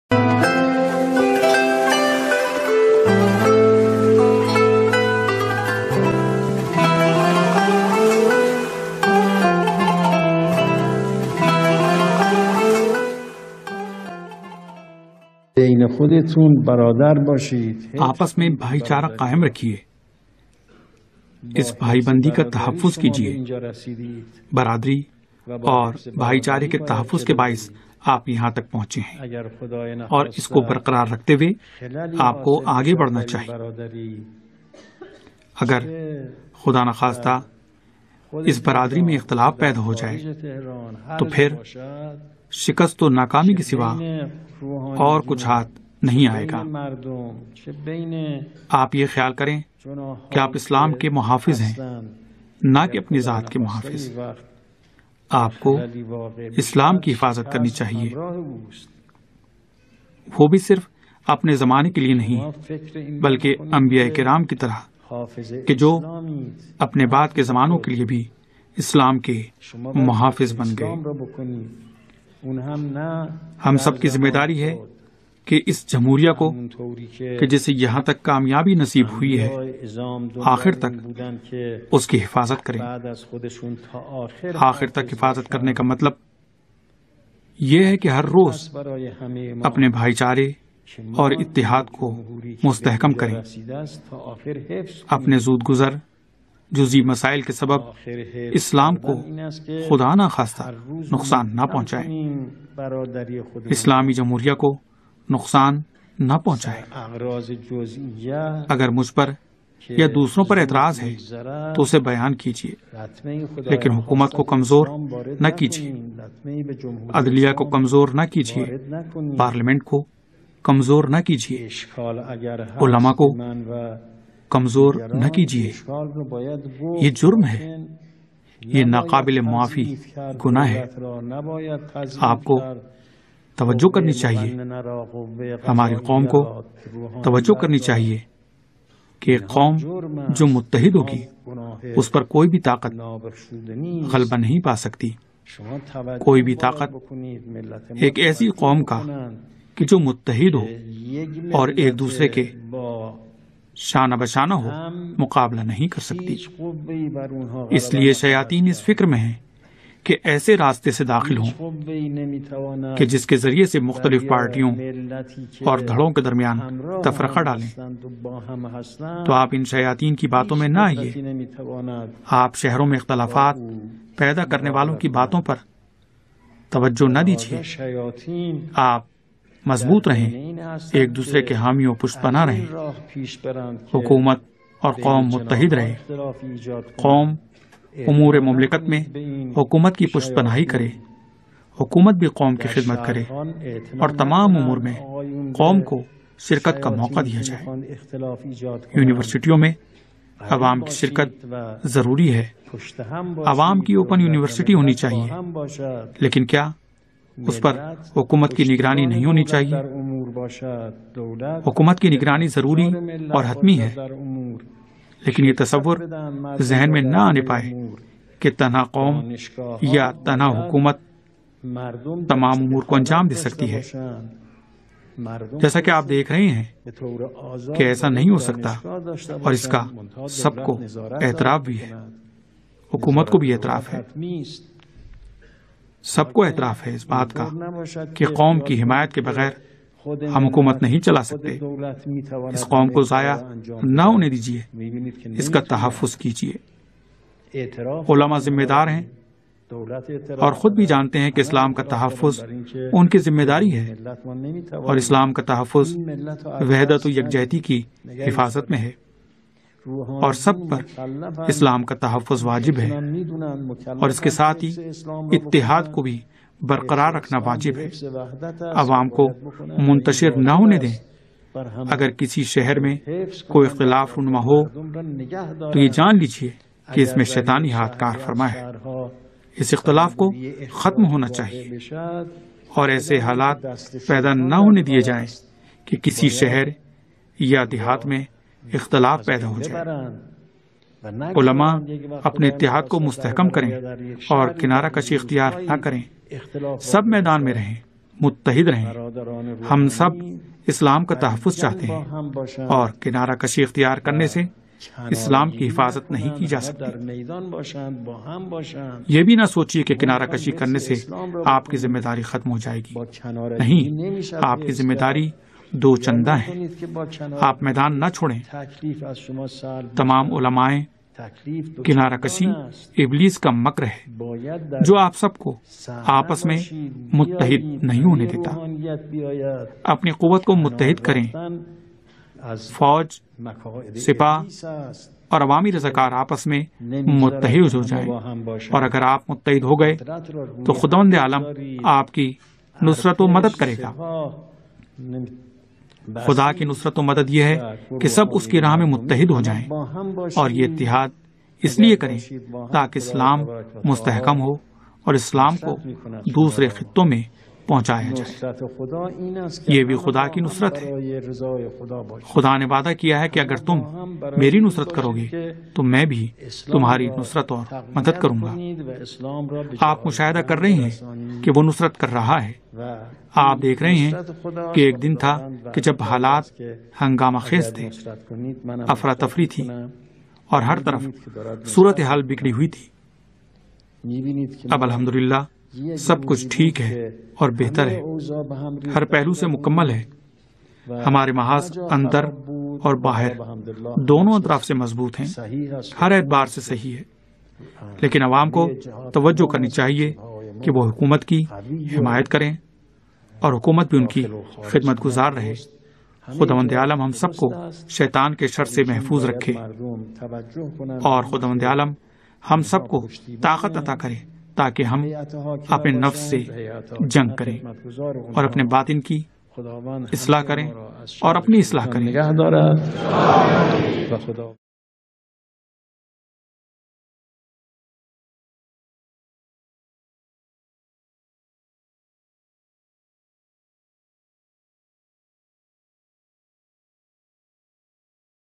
Ithad o yaganyat " آیت اللہ العظمٰی سیدروح اللہ الموسوی خمینیؒ ۔ موضوع :"اتحاد و یگانی
"رمضان مہربانی"
آیت اللہ العظمٰی سیدروح اللہ الموسوی خمینیؒ
موضوع :"اتحاد و یگانیت""
6 رمضان المبارک
پیشکش : نور پروڈکشن پاکستان